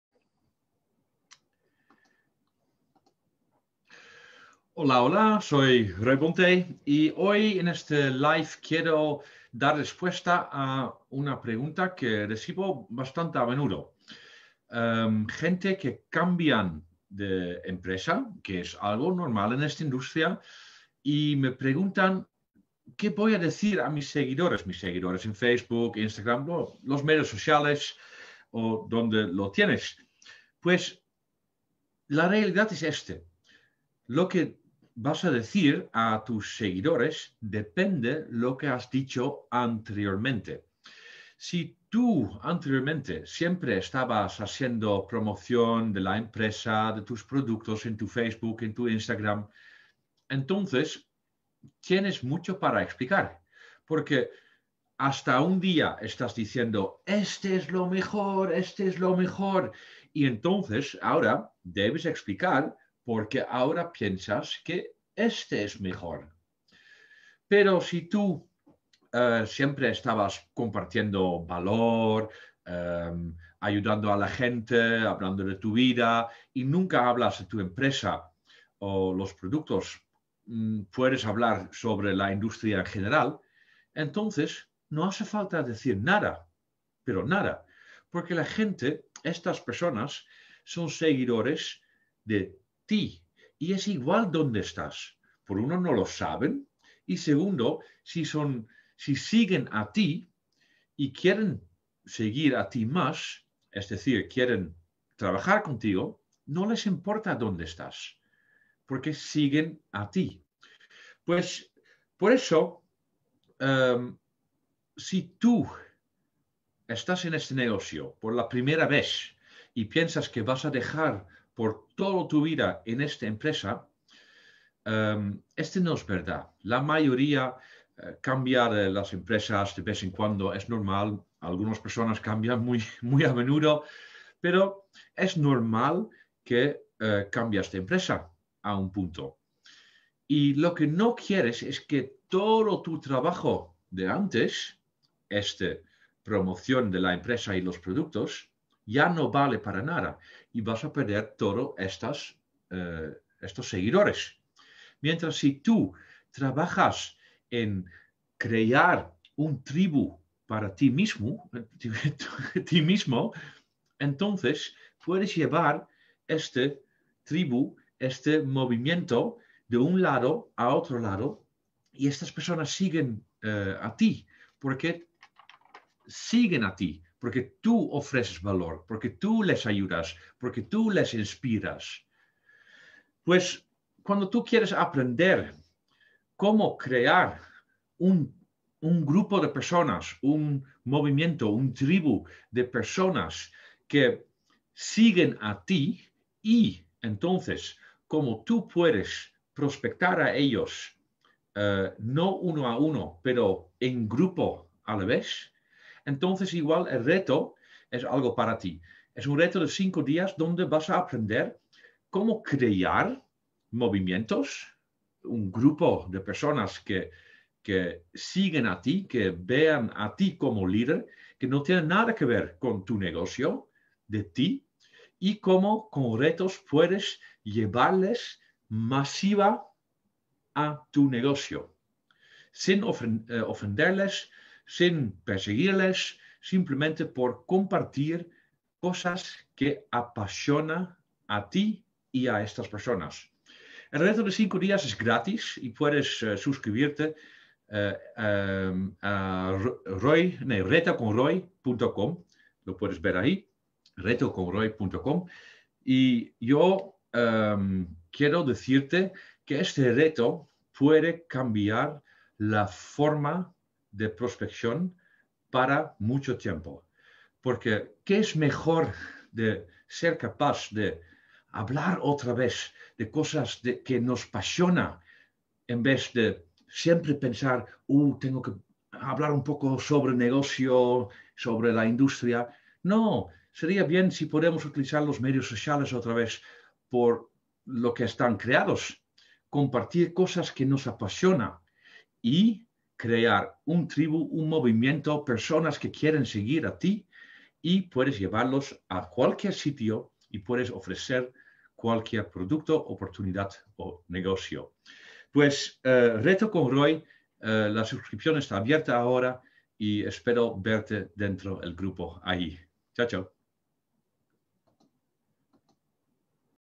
RB ¿Cambiaste de empresa ¿Qué les dices a tus seguidores
Una pregunta que recibo muy a menudo.

He cambiado de empresa.
Que digo a mis seguidores.

Este realmente depende mas en lo que has dicho antes de cambiar empresa.

Mira la formación gratis aquí.

RECORDATORIO
No quieres perder el Reto De 5 Días.
¿No estás dentro todavía?
Regístrate aquí gratis.